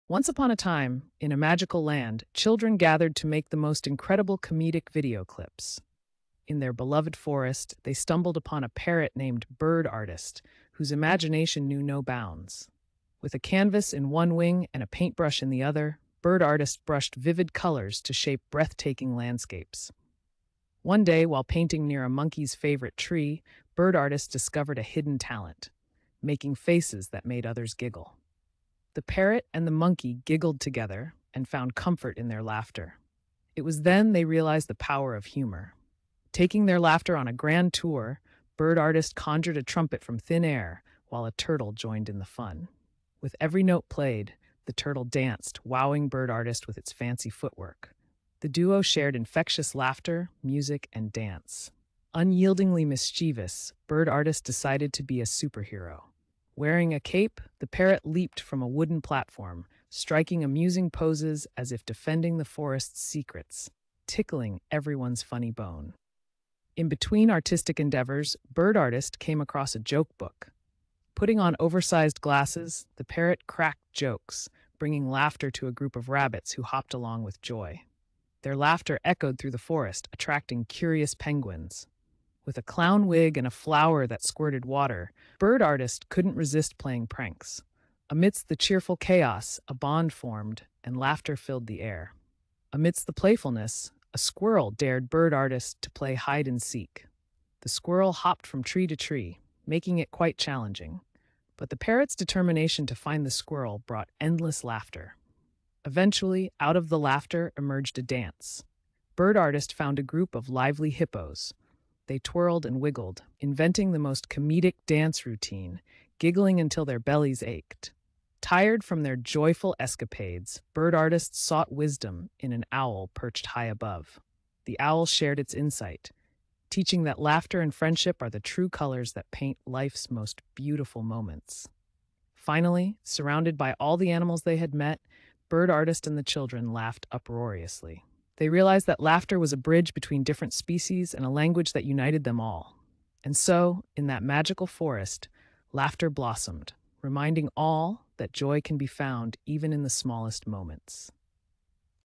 The Hilarious Adventure of Bird Artist
Join the fun as children create comedic video clips with their imaginations and sense of humor. Watch as they bring to life the mischievous parrot, Bird Artist, through a series of hilarious actions. From painting landscapes to dancing with animals, their creativity knows no limits. In the end, they learn an important lesson about the power of laughter and friendship. Don't miss this heartwarming and funny journey! The parrot Bird